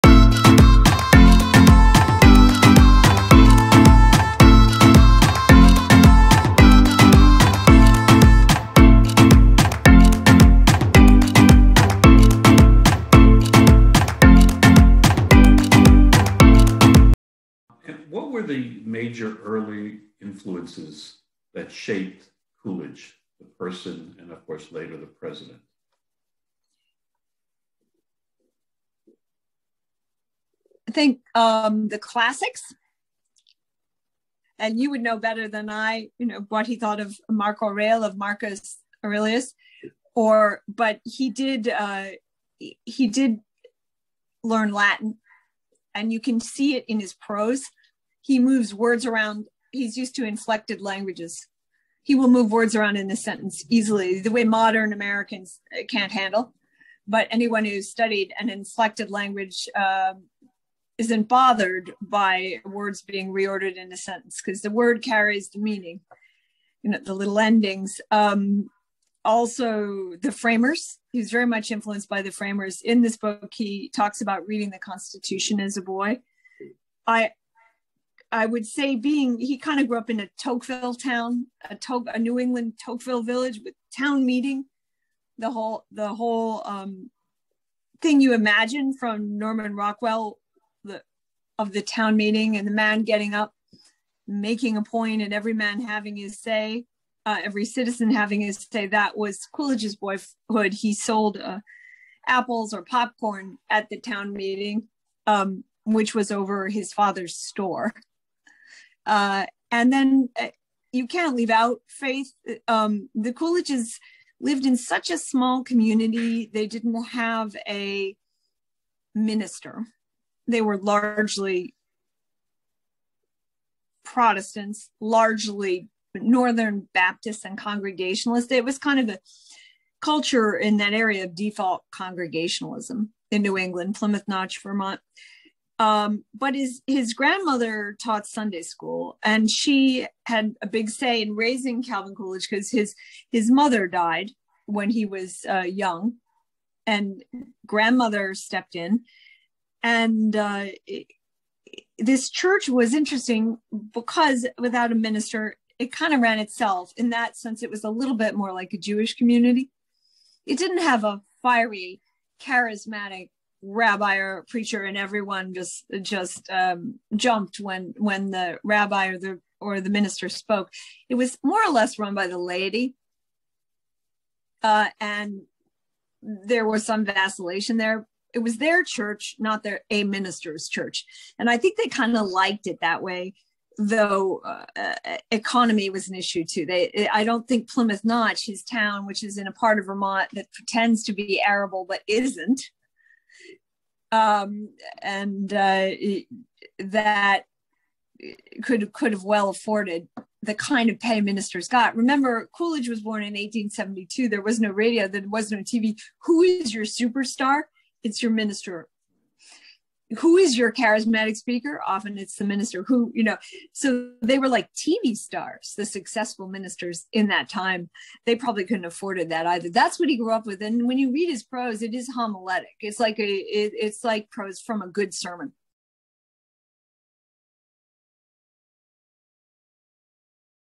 What were the influences that shaped Coolidge?
Join Amity Shales, Chairperson of the board of the Calvin Coolidge Presidential Foundation
Presidential Scholar at the King's College,
And, Award Winning Author, of four New York Times bestsellers:
The Forgotten Man: A New History of the Great Depression,
The Forgotten Man: Graphic
The Greedy Hand: How Taxes Drive Americas Crazy.
Calvin Coolidge

as she presents her penetrating insights into the life and presidency of Calvin Coolidge.

"To read Amity Shlaes's well-crafted biography is to understand why Reagan so admired the famously reticent man whom Shlaes calls 'our great refrainer.'" (George F. Will)

• The Classics; Coolidge learned Latin
• Framers; Coolidge read the constitution as a boy
• New England background
• Faith – church but without a minister
• Grandmother helped raise him after mother’s death